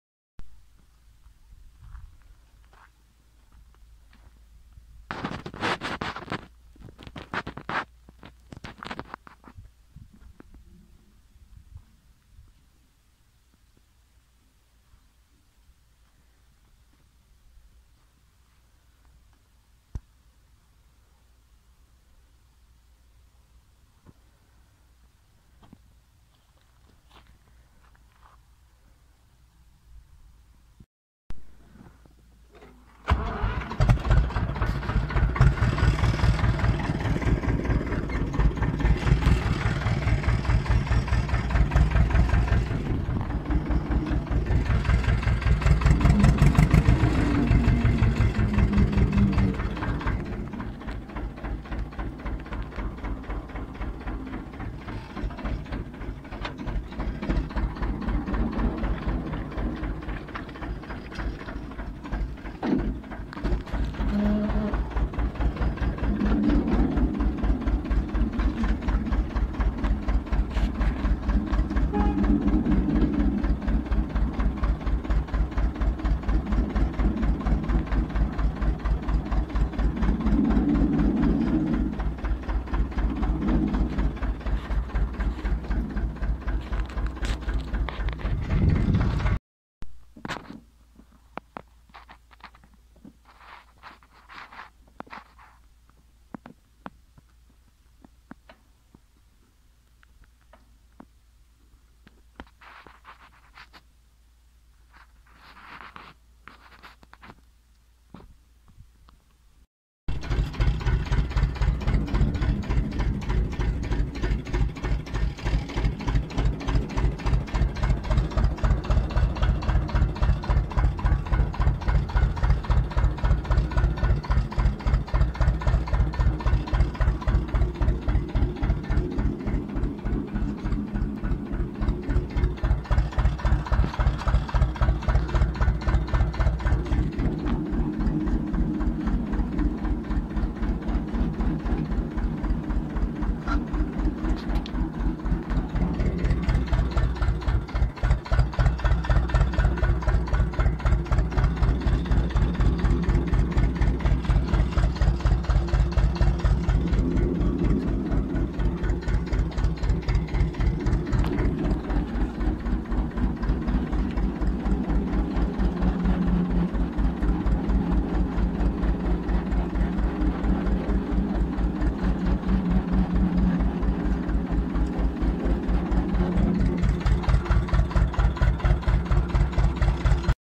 Mini wtopa😂🤣 przy pieleniu kalafiora=Ursus c 330 M i lekki wyjazd (opis)
Witam dzisaj taki film nic innego nie miałem bo dopiero muszę zrobić film który był nagrywany kamera a nie telefonem ale z względu na czas którego ciągle jest mało to nie wyrobiłem się a już w niedzielę vlog z kopru do kieszeni pozdrawiam

W filmie wystąpili:
Ursus c 330 m
Akpil 3 sekcyjny